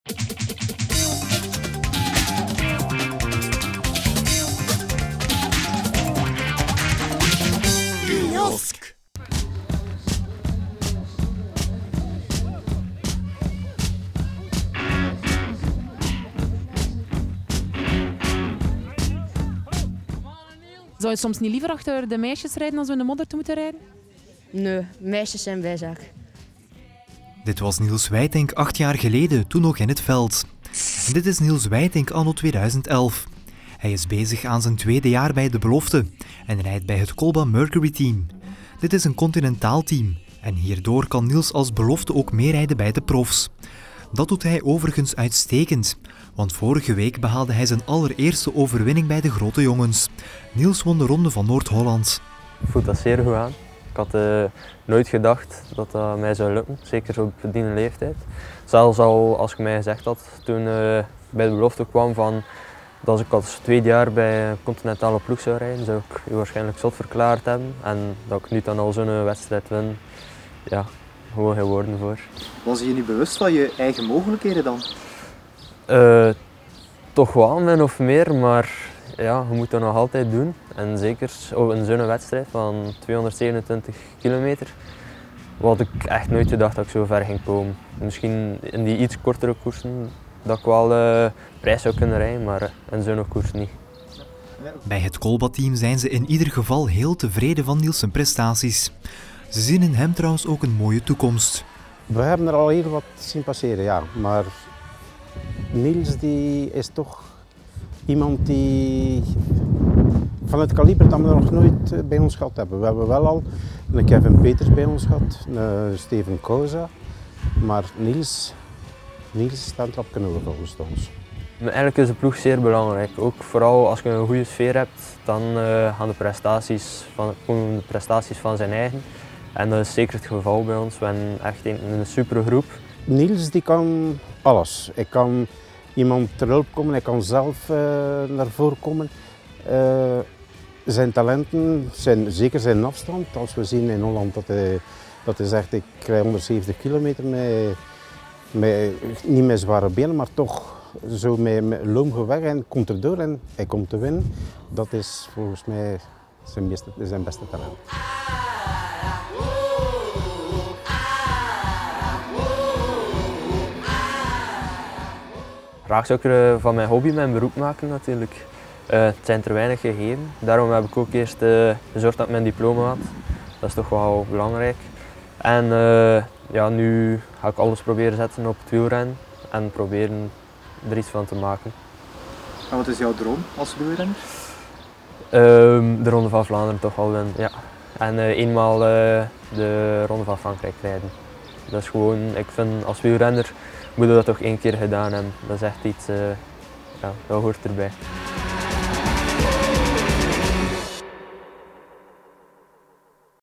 AVS Kiosk Niels Wytinck
Lembekenaar Niels Wytinck scheert momenteel hoge toppen in zijn tweede jaar als belofterenner. Hij won zelfs al een eerste profkoers: de Ronde van Noord-Holland.